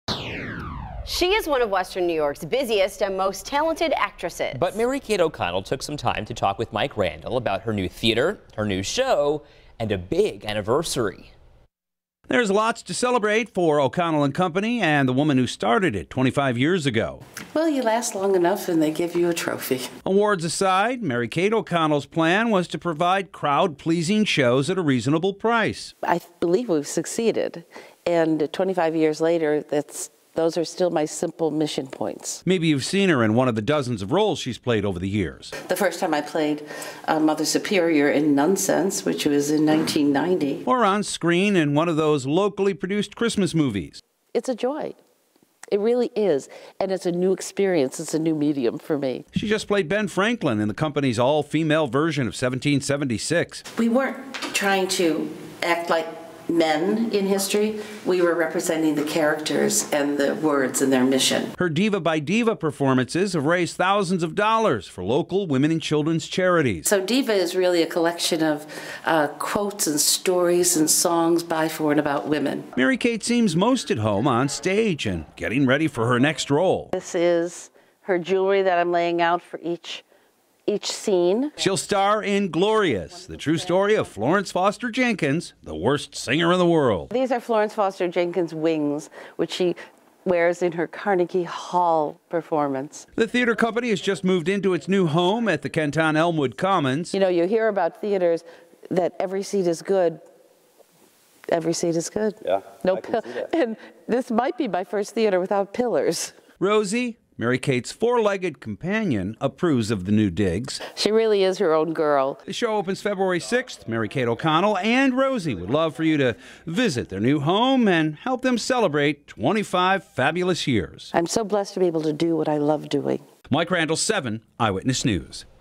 Celebrating a new home, new show and 25 years of O'Connell &amp; Company
Mary Kate O'Connell started it all 25 years ago